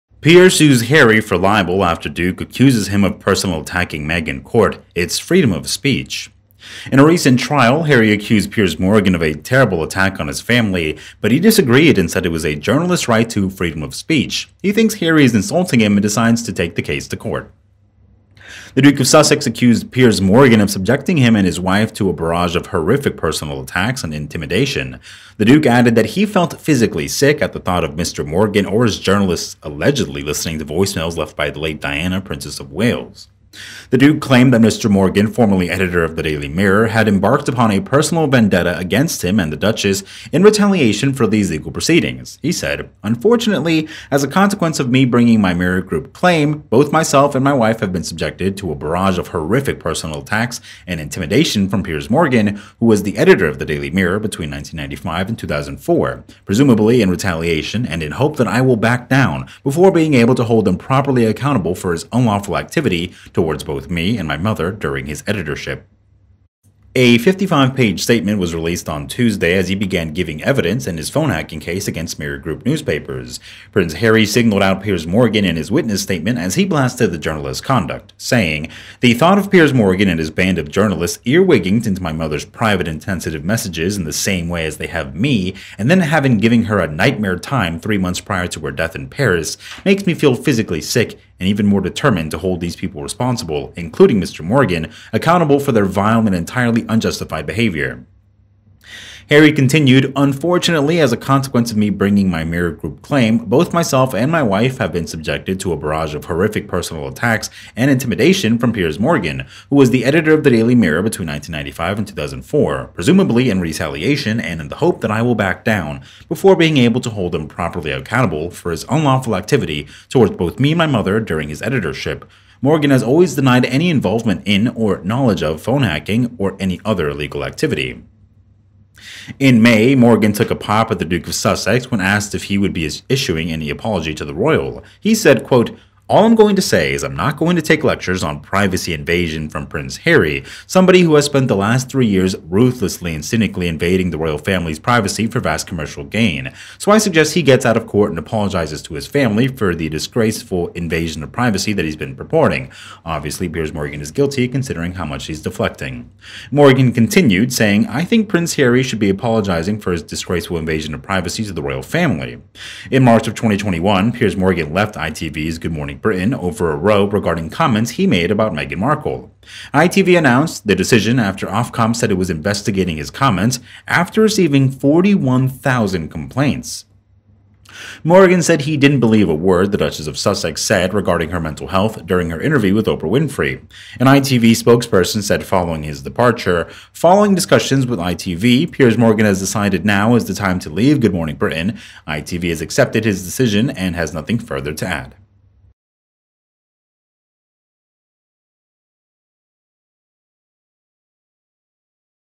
Pier Sue Haz For Libel After Duke Accuse Him Of Personal Attack Meg In Court:It’s Freedom Of Speech
In a recent trial, Harry accused Pier Morgan of a terrible attack on his family, but he disagreed and said it was a journalist's right to freedom of speech. He thinks Harry is insulting him and decides to take the case to court